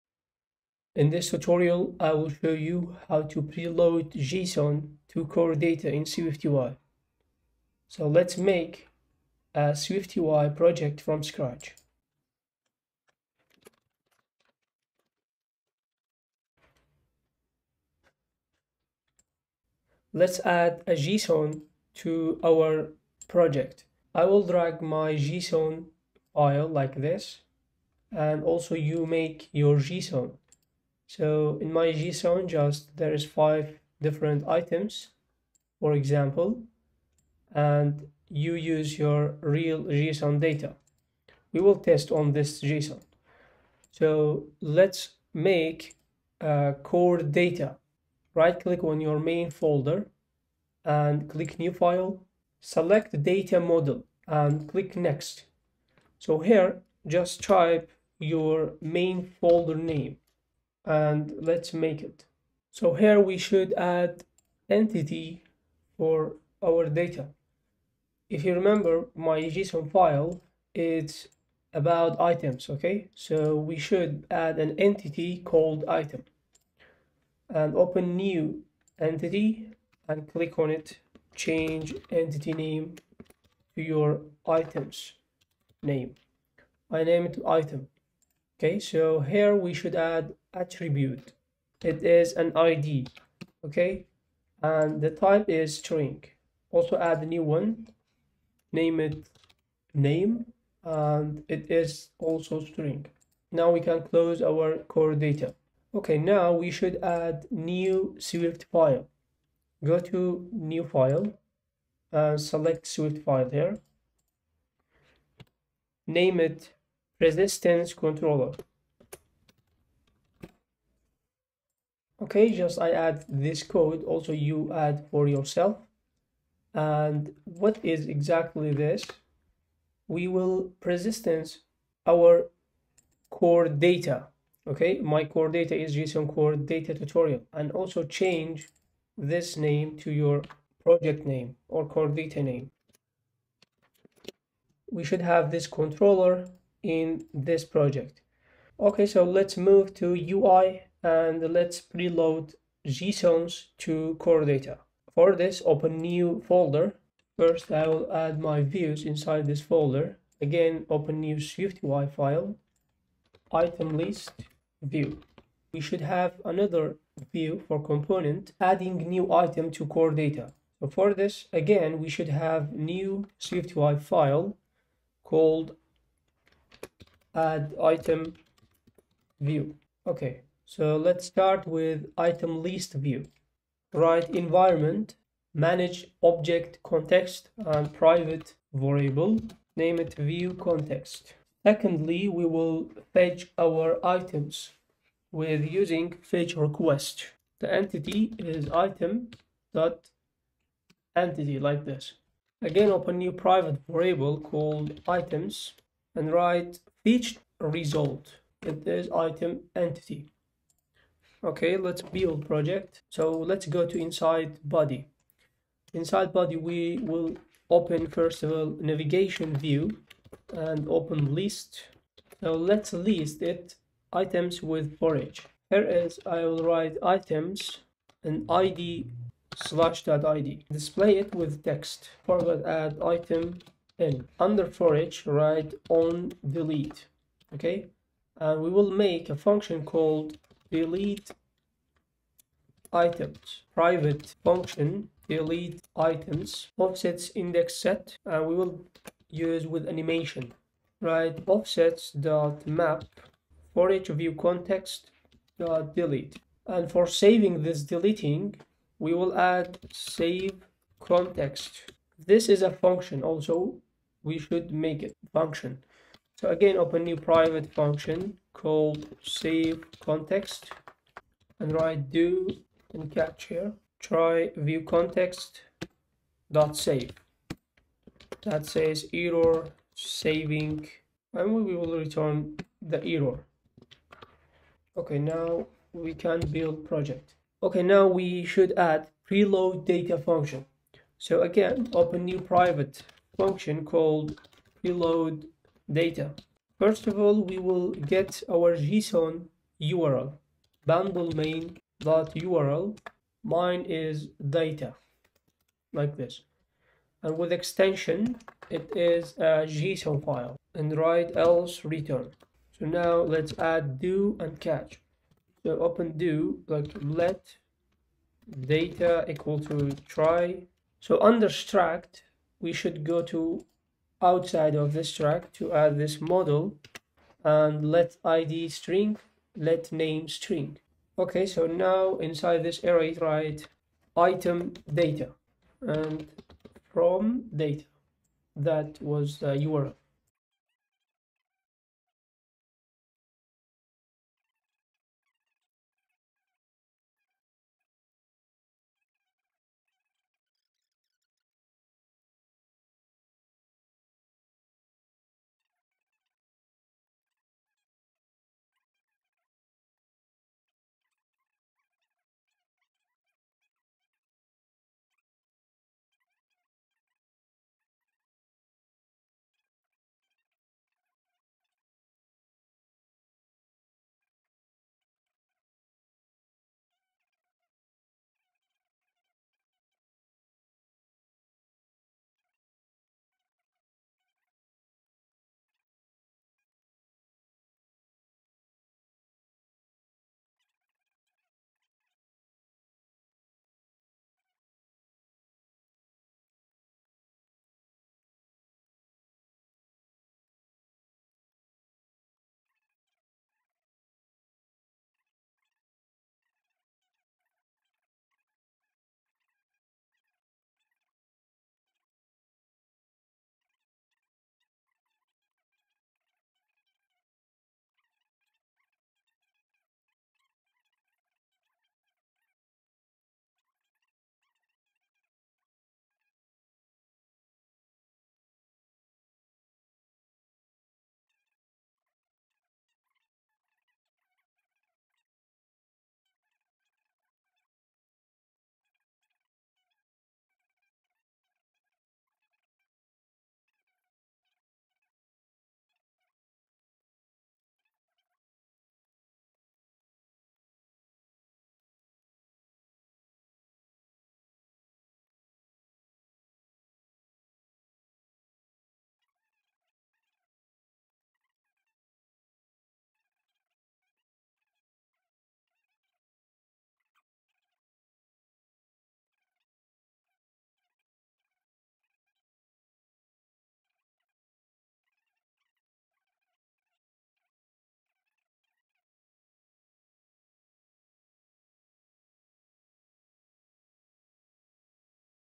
Preload Data from JSON File and Map it to Core Data | SwiftUI Tutorials
in this tutorial i will show you how to preload data from json and map it to Core data.